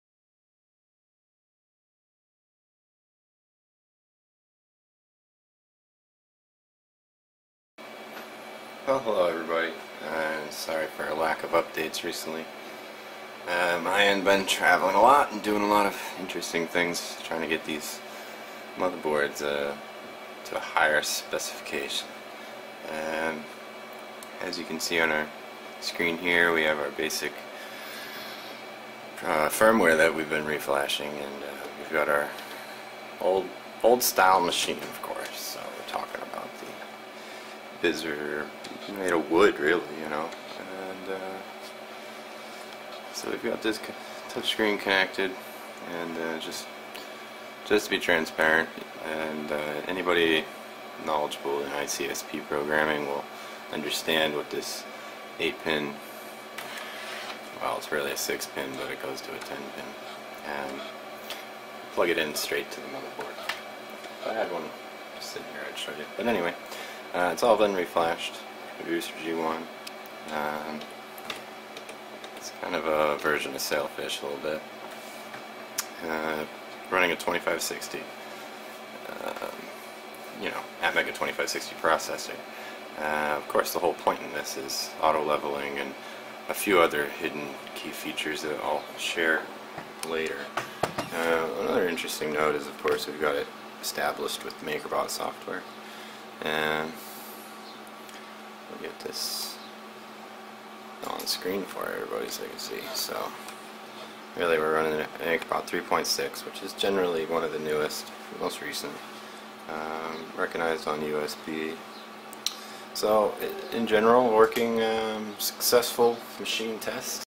Vlog 2: Makerbot Atmega 2560 test w/USB
Test of 2560 Processor on Makerbot 3D Printer motherboard.

-Touchscreen with interface connected, and power from motherboard.
-Custom Firmware